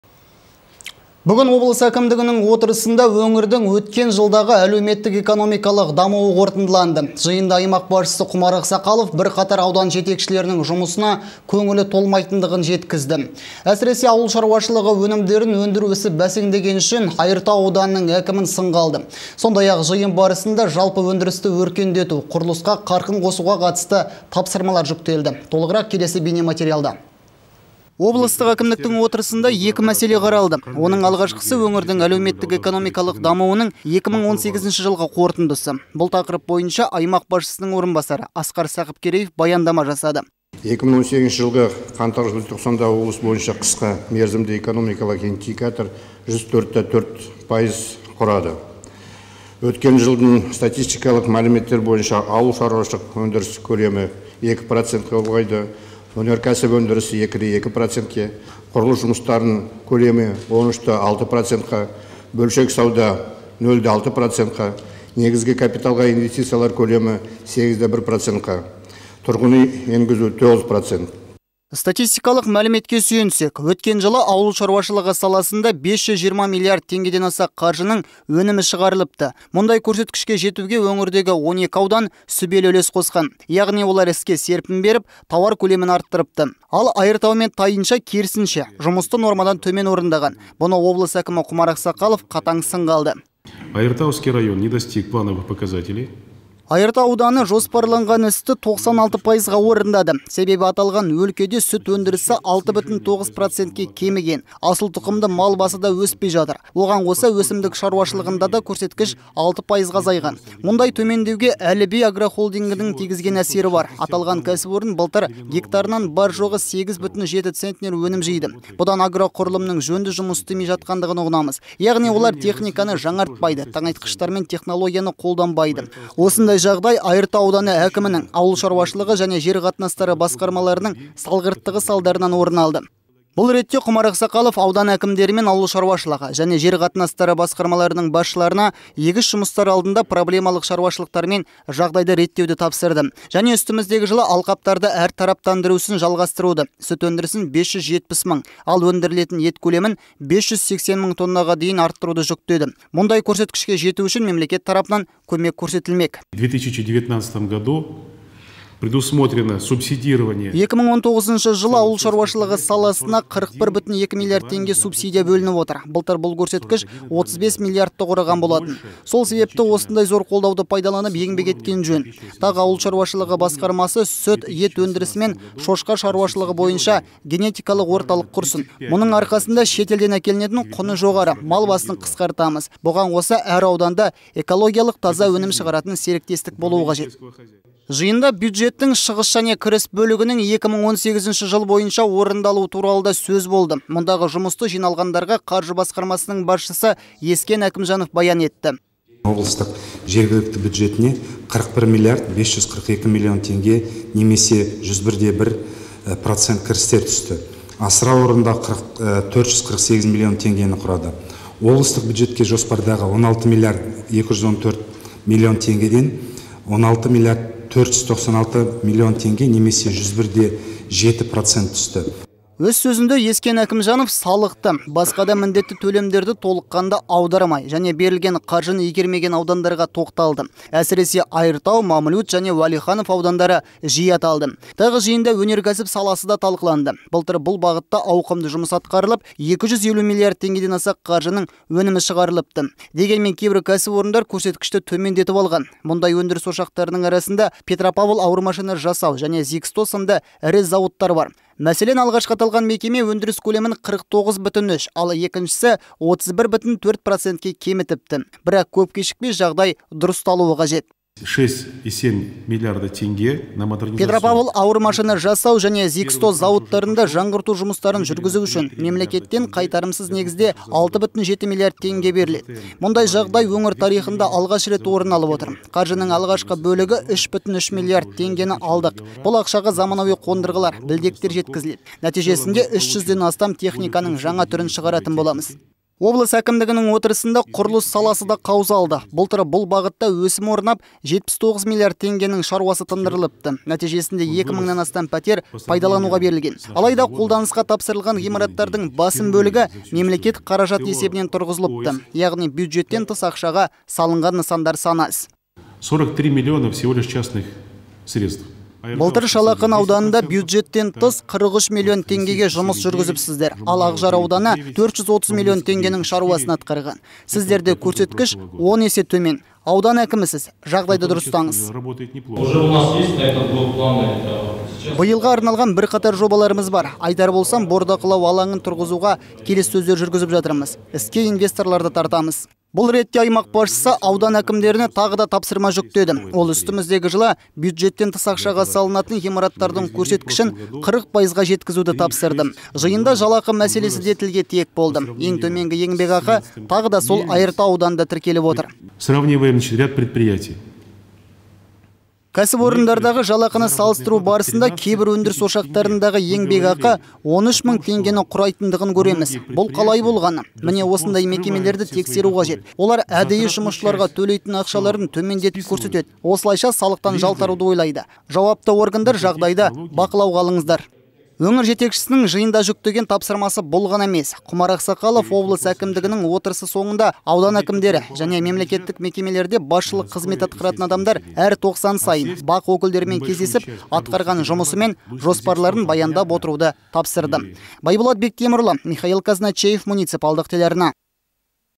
Өңірдің өткен жылға әлеуметтік-экономикалық дамуы
Бүгін облыс әкімдігінің отырысында өңірдің өткен жылдағы әлеуметтік-экономикалық дамуы қорытындыланды. Жиында аймақ басшысы Құмар Ақсақалов бірқатар аудан жетекшілерінің жұмысына көңілі толмайтындығын жеткізді. Әсіресе, ауыл шаруашылығы өнімдерін өндіру ісі бәсеңдегені үшін Айыртау ауданының әкімін сынға алды. Сондай-ақ, жиын барысында жалпы өндірісті өркендету, құрылысқа қарқын қосуға қатысты тапсырмалар жүктелді. Толығырақ келесі бейнематериалда.